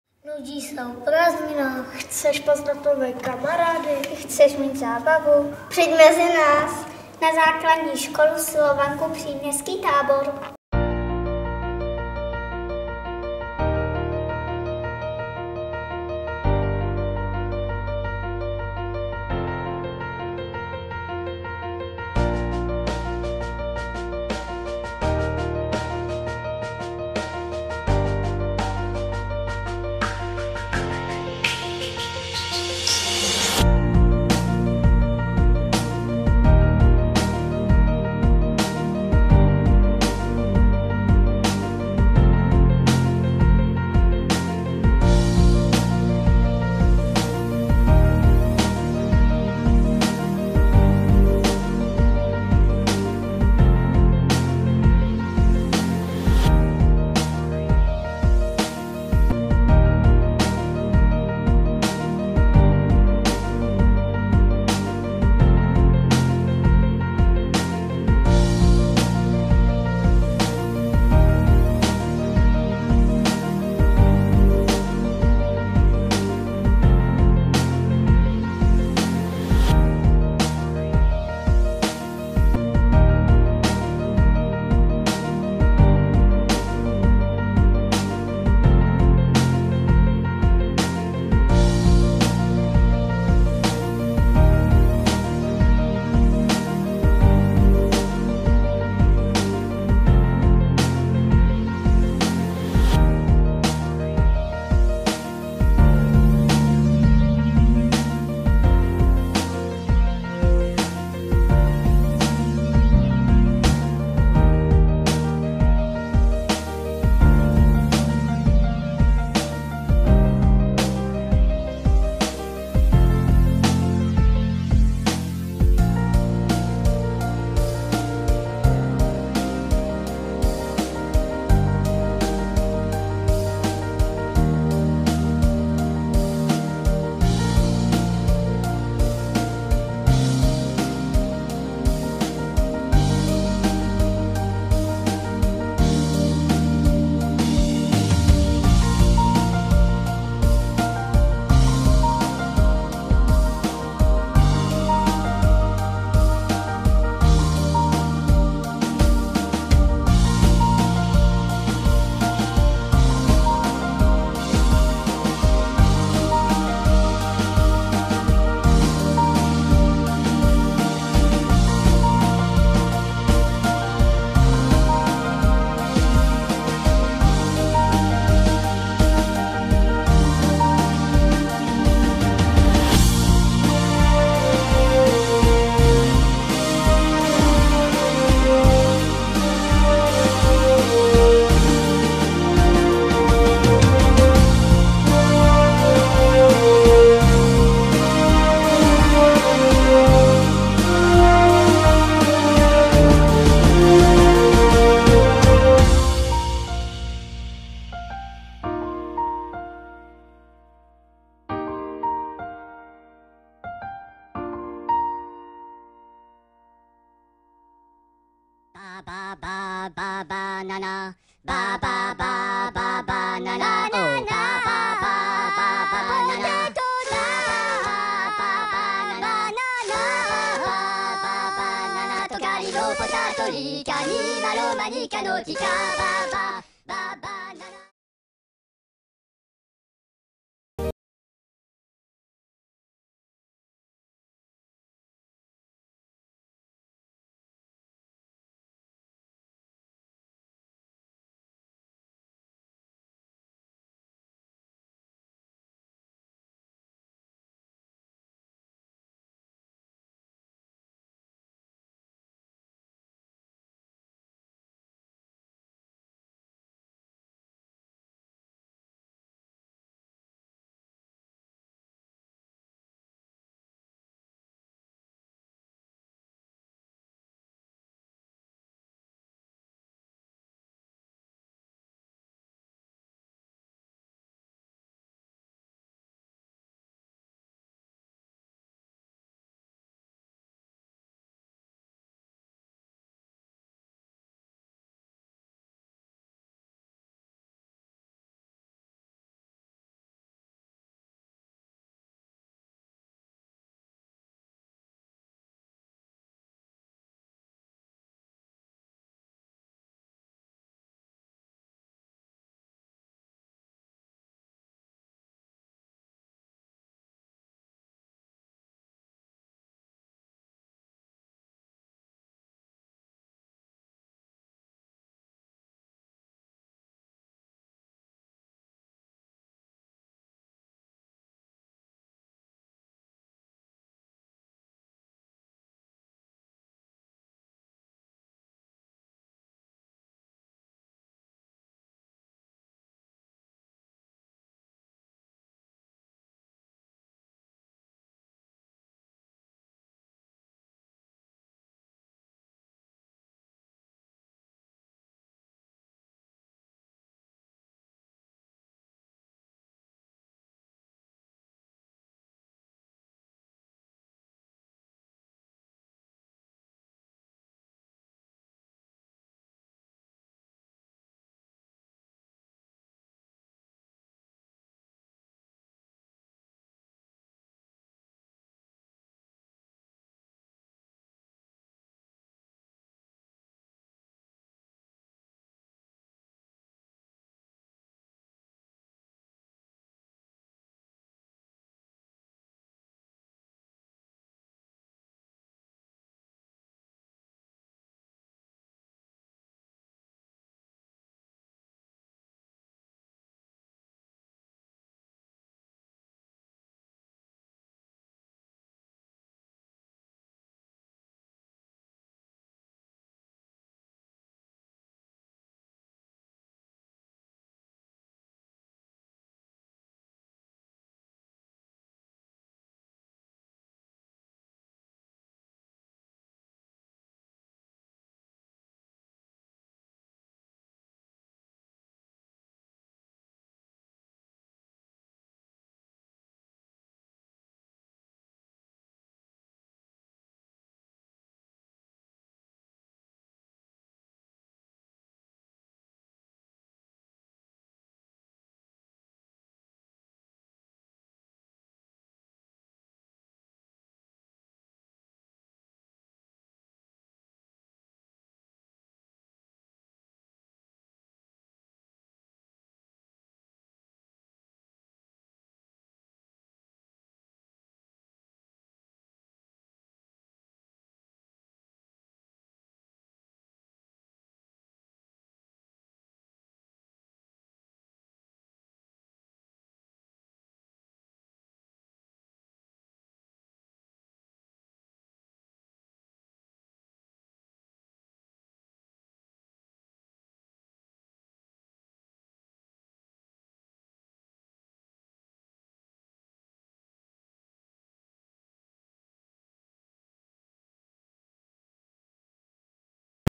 Příměstský tábor ZŠ Slovanka
Ohlédnutí za prvním měsícem příměstského táboru na ZŠ Slovanka v České Lípě a současně pozvánka na měsíc následující. Stále se ještě můžete hlásit na
Zdraví vás a těší se na vás vaši vedoucí: Terka s Áďou, Vašek s Káťou, Jirka s Jiřkou a Klárka s Terkou.